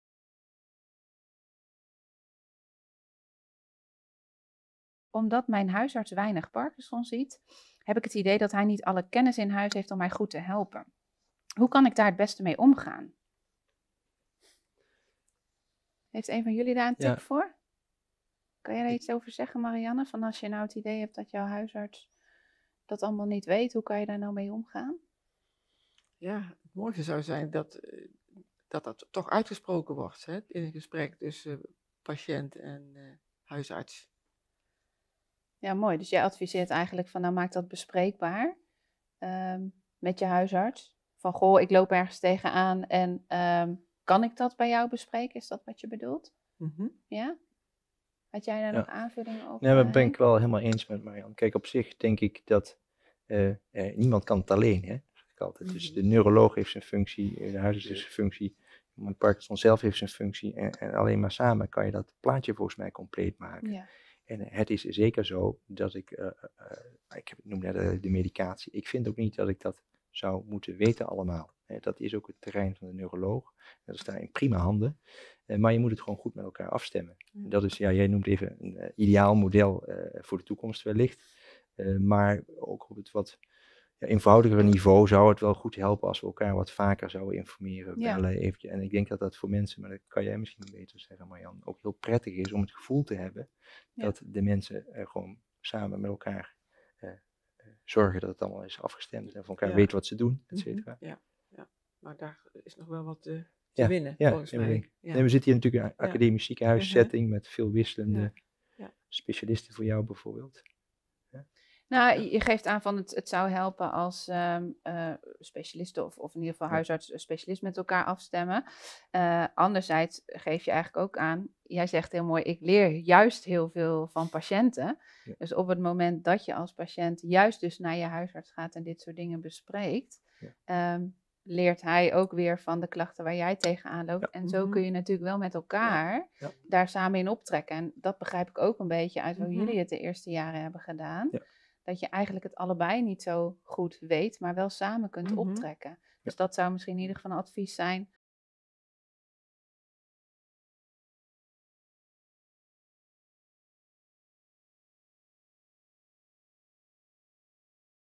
Mijn huisarts ziet weinig mensen met parkinson, heeft hij wel genoeg kennis?
Omdat mijn huisarts weinig parkinsonpatiënten ziet, heb ik het idee dat hij niet alles kennis in huis heeft om mij goed te helpen.